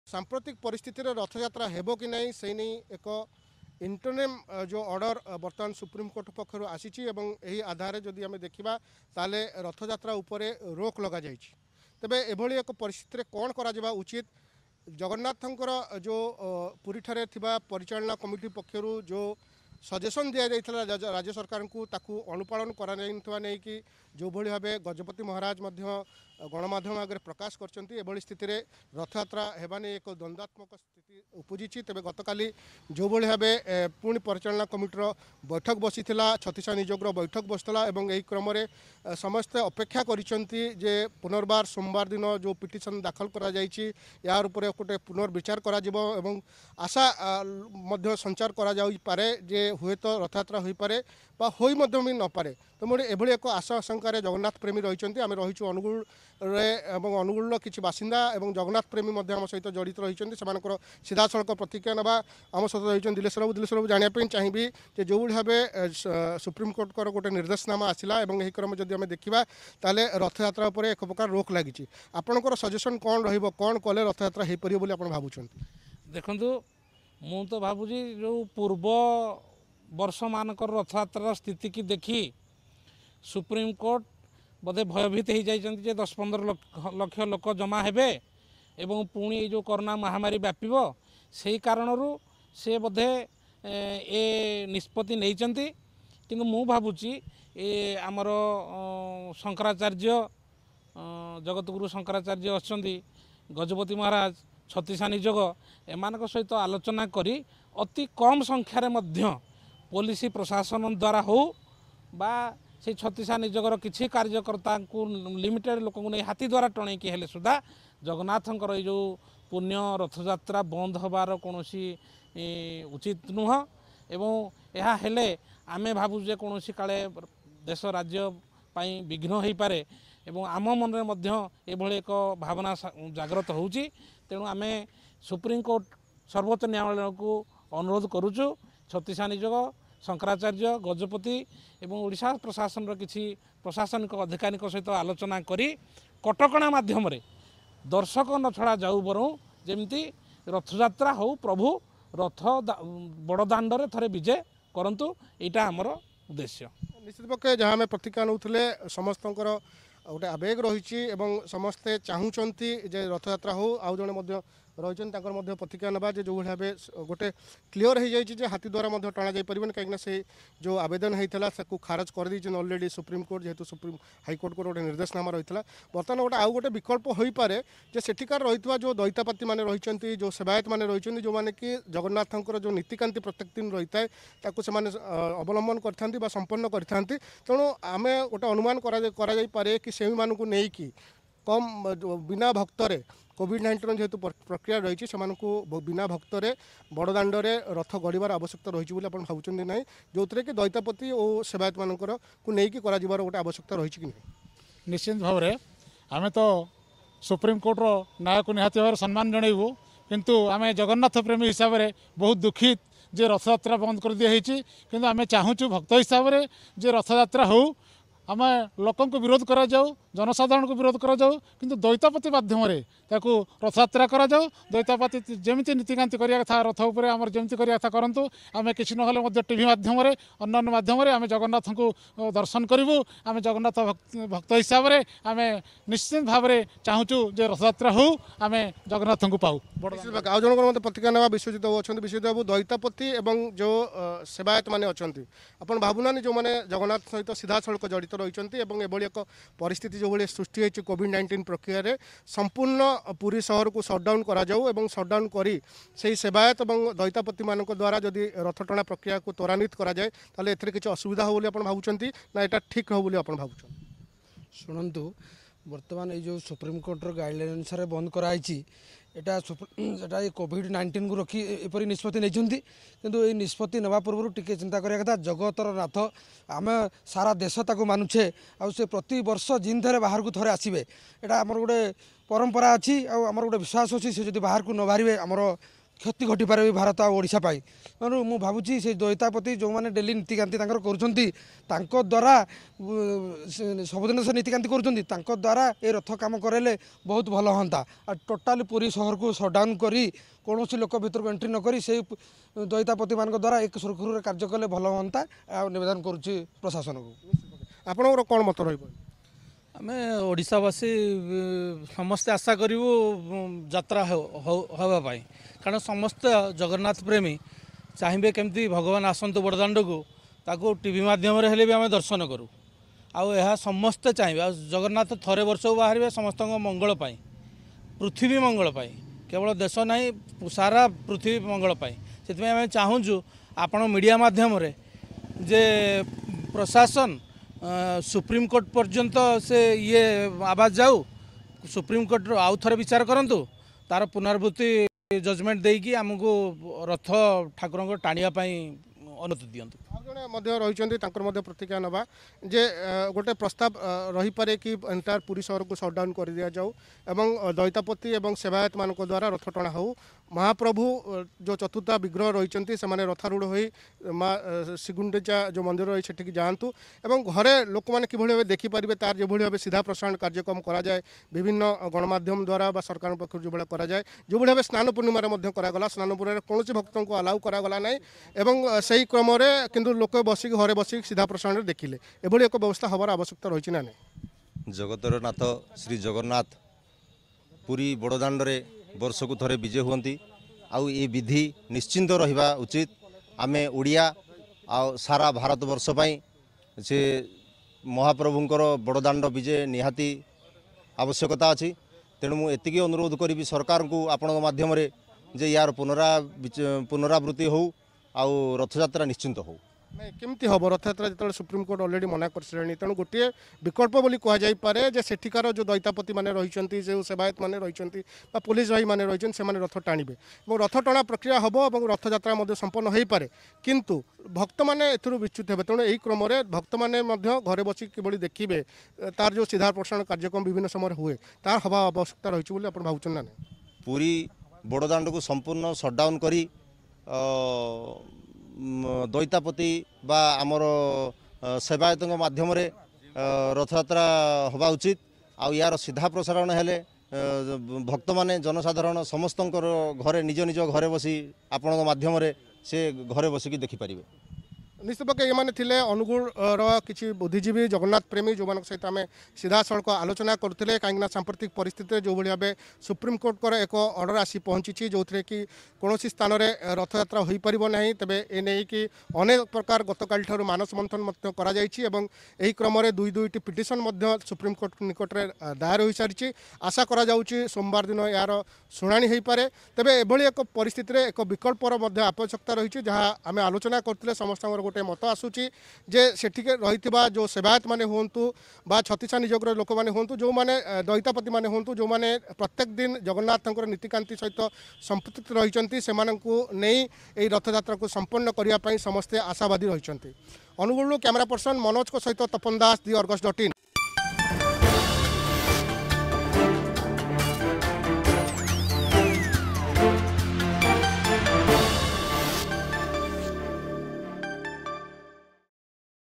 ରଥଯାତ୍ରା ବିବାଦ .....
ରଥଯାତ୍ରା ବିବାଦ । ସୁପ୍ରିମକୋର୍ଟରେ ମୋଡିଫିକେସନ ପାଇଁ ଆଜି ଦୁଇଟି ପିଟିସନ ଦାୟର ହୋଇଛି । କେବଳ ପୁରୀରେ ରଥଯାତ୍ରା ପାଇଁ ଅପିଲ ହୋଇଛି । ଅନ୍ୟପକ୍ଷେ ଛତିଶାନିଯୋଗ , ଗଜପତି ମହାରାଜ ଓ ଶଙ୍କାରାଚାର୍ଯ୍ୟଙ୍କ ପରାମର୍ଶ କ୍ରମେ ଓ କଟକଣା ସହିତ ରଥଯାତ୍ରା ପାଳନ କରିବାକୁ ମତ ରଖୁଛନ୍ତି ଅନୁଗୁଳ ଜଗନ୍ନାଥ ପ୍ରେମୀ.